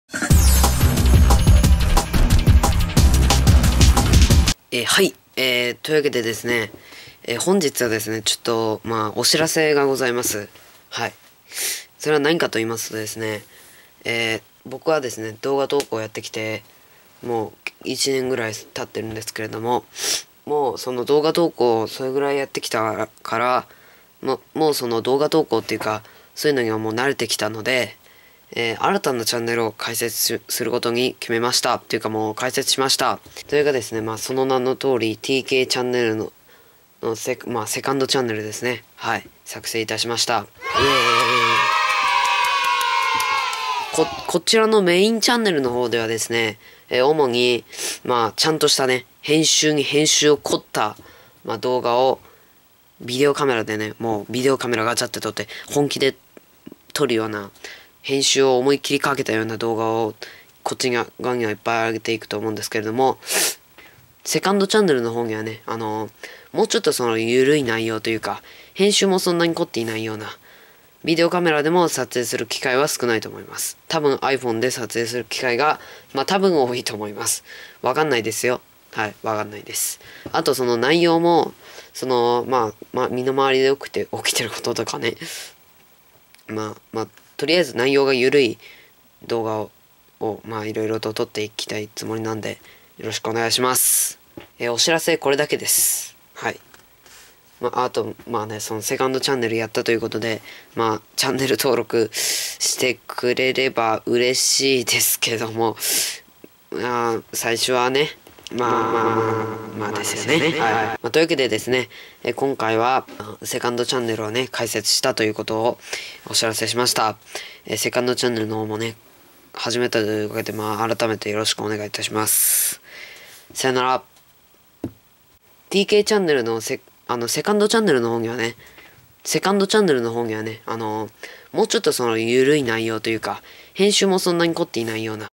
2ndチャンネル始めました。｜SubChannel Start!
本日もTKChannelの動画をご視聴いただき誠にありがとうございます。

今日はですねサブチャンネルを開設したことをお知らせします。

また動画が見­­­­­­­­­やすくなるのでぜひお願­­­いします。高評価も忘れずにっ！

本作品は特定の人物や物事の誹謗・中傷を目的として作られたものではありません。

TKChannelをいろんな人と共有しよっ！